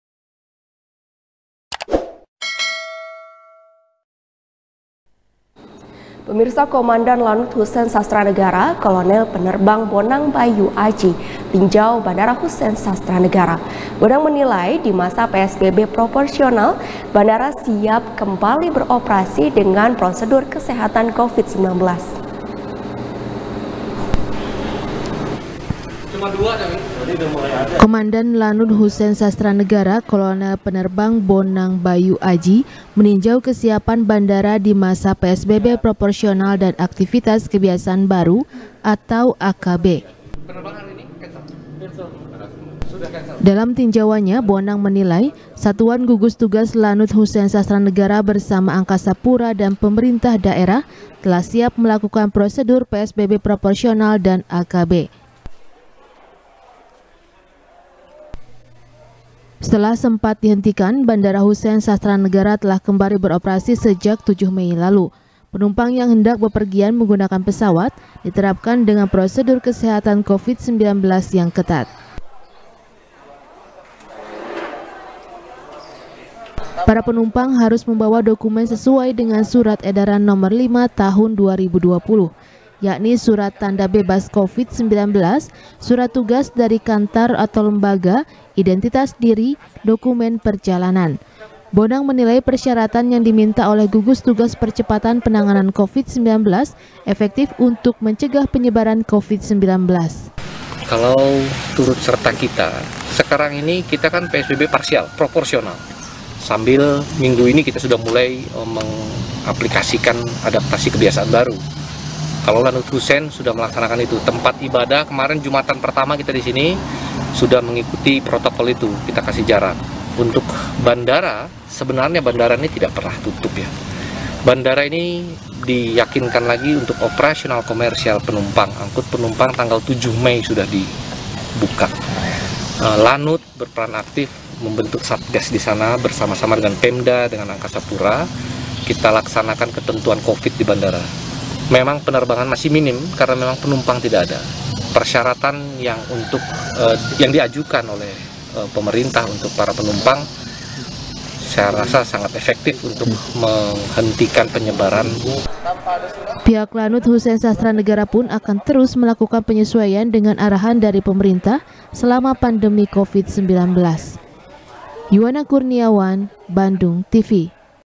Danlanud Tinjau Bandara | SBR | BANDUNGTV
SBR | BANDUNGTV | HANY SHANDRA | Komandan Lanud Husen Sastranegara--Kolonel Pernerbang Bonang Bayu Aji, tinjau Bandara Husen Sastranegara. Bonang menilai di masa PSBB Proporsional, bandara siap kembali beroprasi dengan prosedur kesehatan Covid-19.